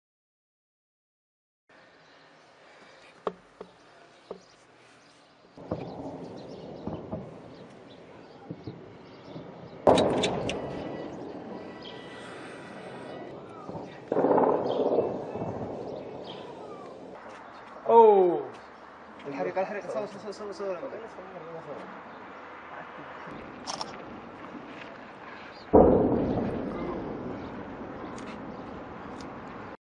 Yémen: les frappes aériennes sur Sanaa continuaient vendredi
La coalition menée par l'Arabie Saoudite, a continué vendredi ses frappes aériennes autour de la capitale, Sanaa. Durée: 00:30